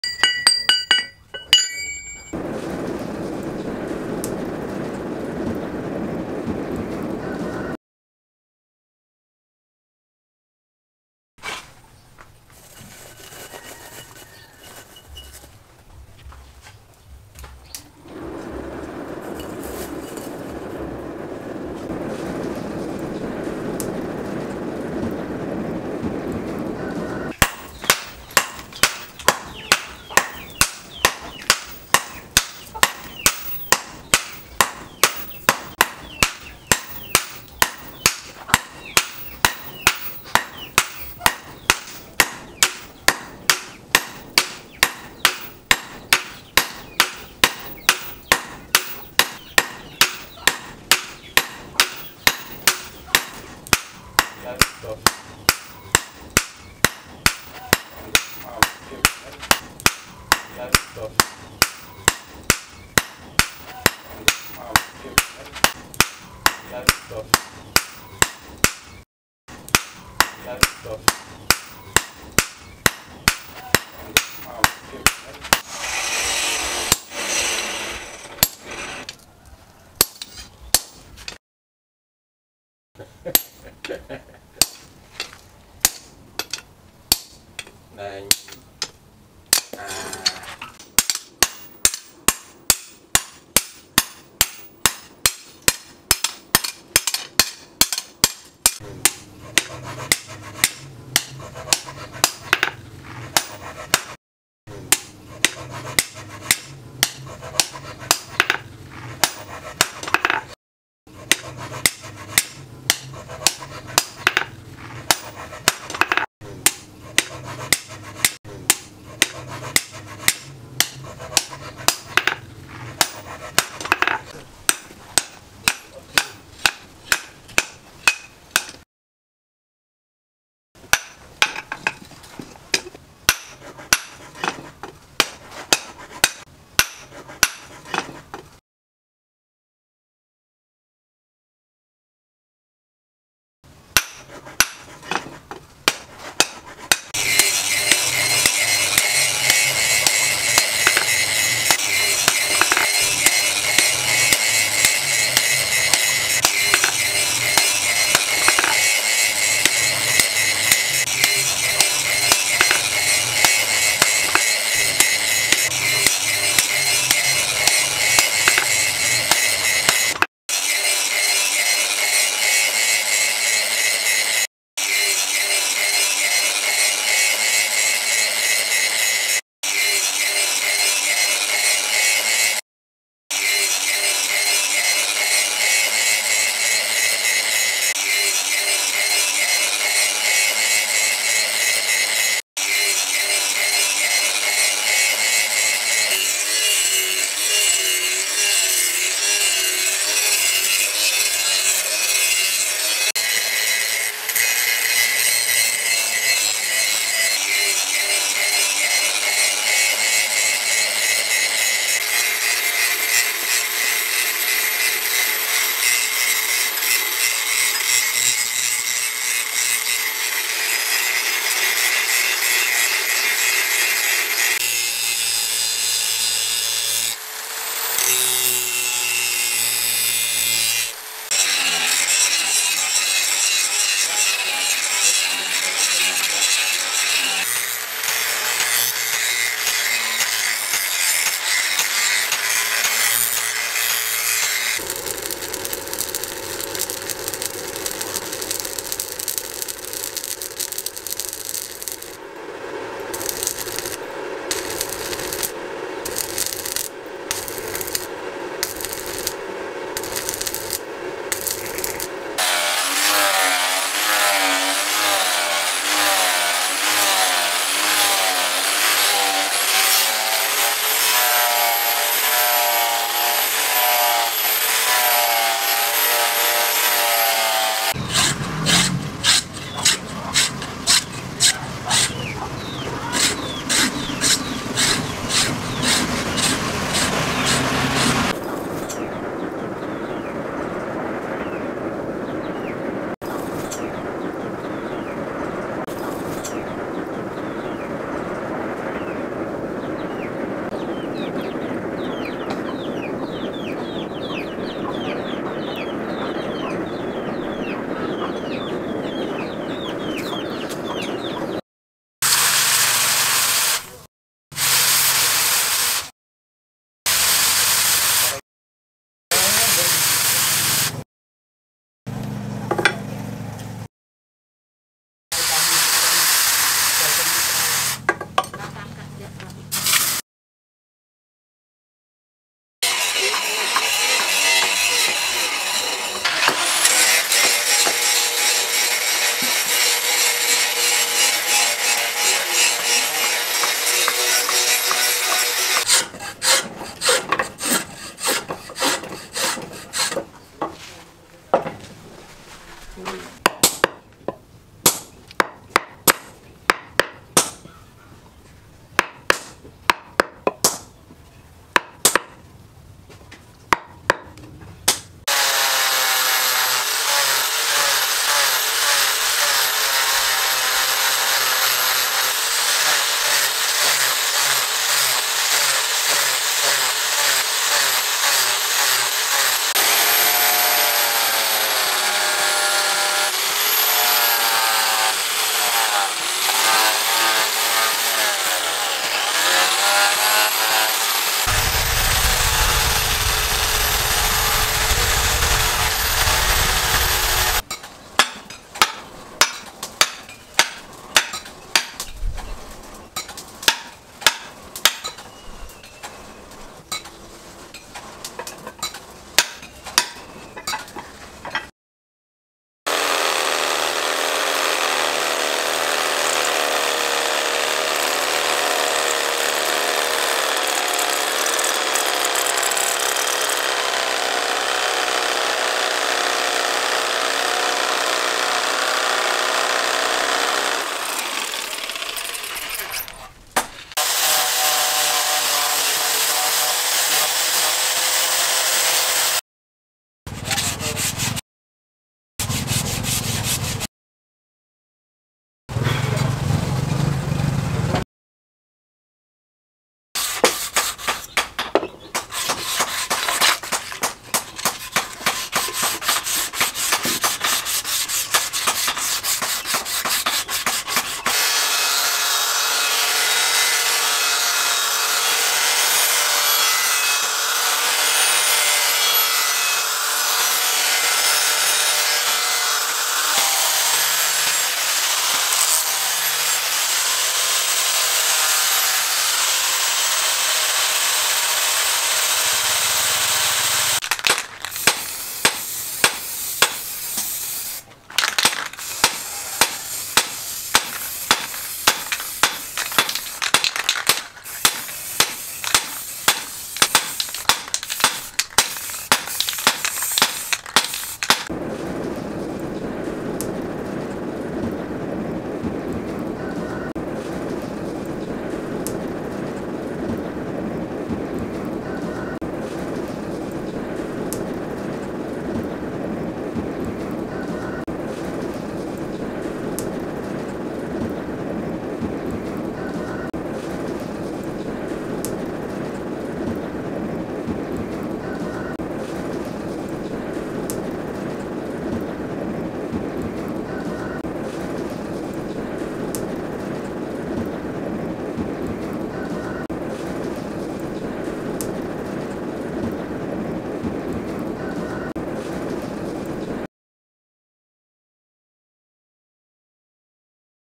Axe Making-Craftsman Blacksmith Makes A Nice Axe.
Today he is making a nice axe from a piece of leaf spring. It is very simple but it is the first time for him. Its handle is made from long wooden.